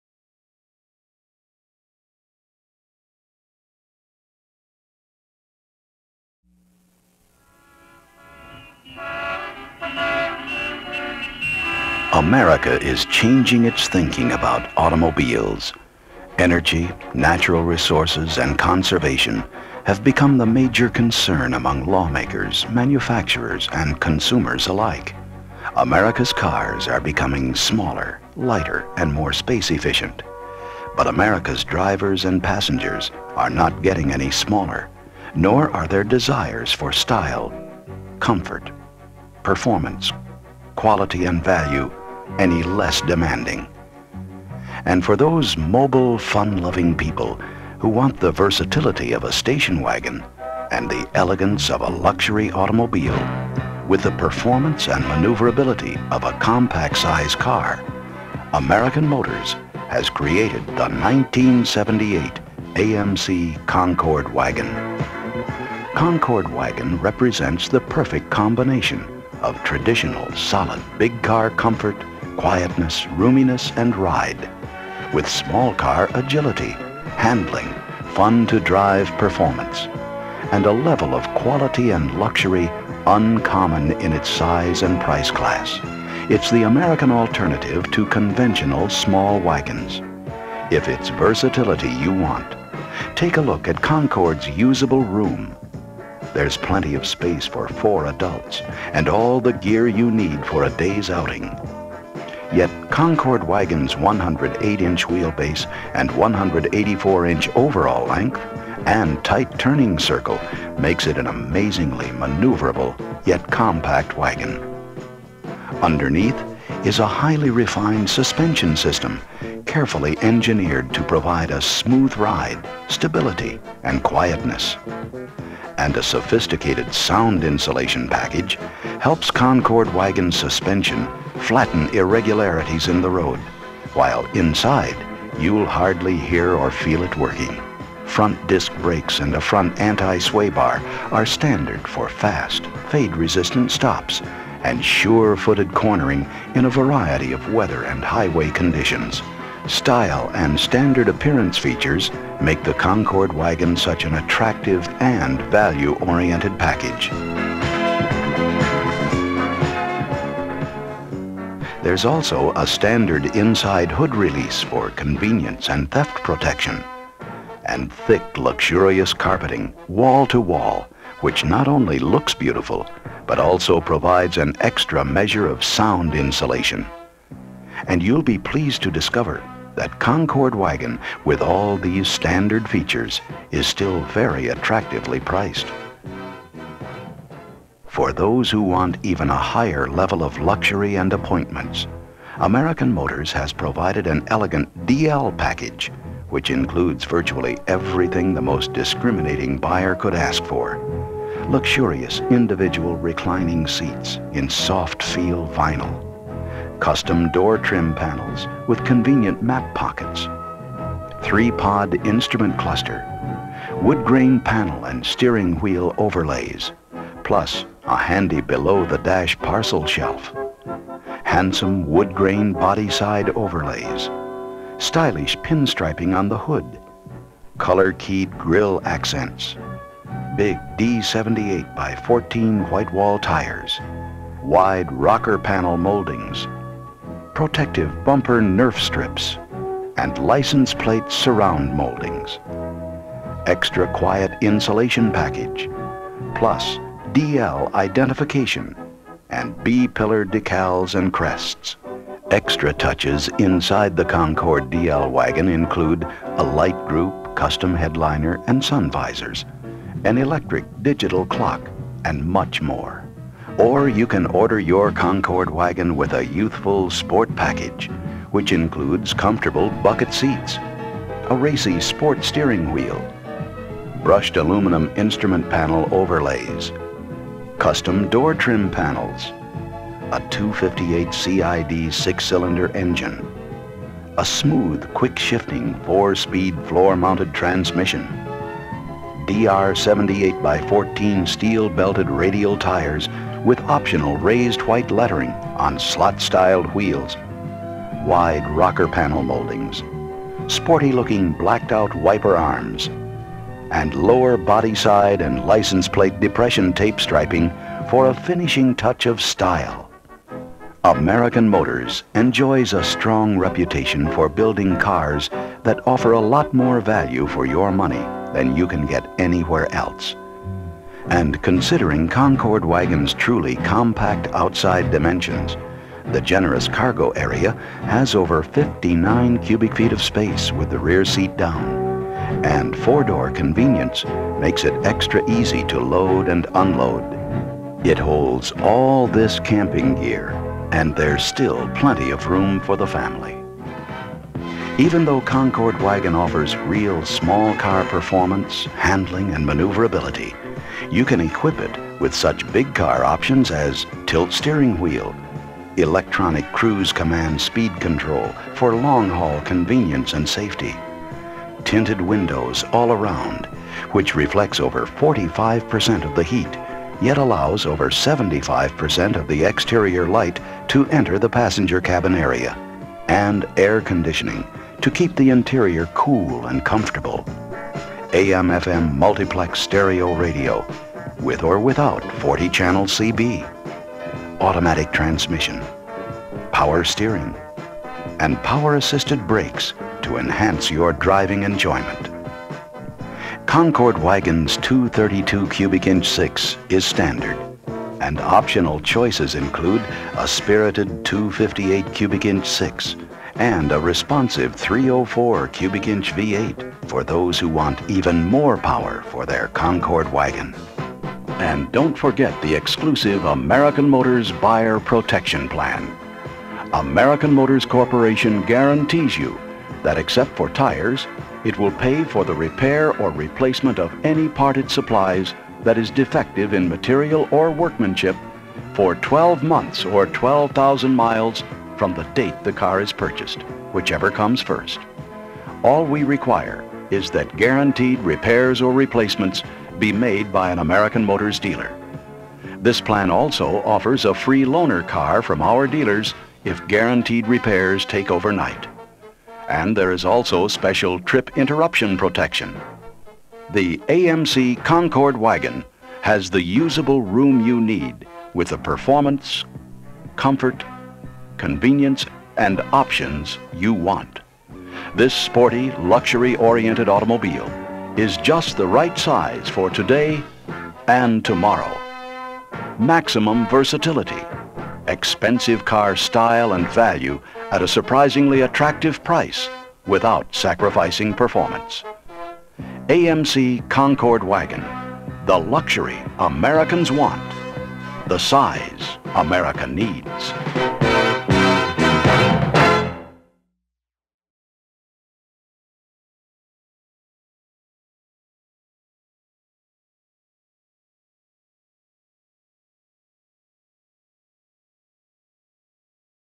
1978 Concord Wagon Product Information
The most prominent features of the 1978 AMC Concord Wagon, meant for the average consumer who may be in the dealership. There are no characters for this video, only the voice of the announcer to accompany the visuals. This presentation is different from the regular Concord presentation, also available on this channel.

The Sentinel 16 was a combination 8-track cartridge player and automated filmstrip projector, used in the mid-1970's. The cartridges, which contained both the film and the audio tape, were distributed to AMC dealers around the nation. Our exclusive restoration process, constructed by Craig Patterson, shows you the machine display exactly as it was originally meant to be seen. Colors have been restored as closely as possible, though the filmstrips were of questionable quality, and have severely degraded over time.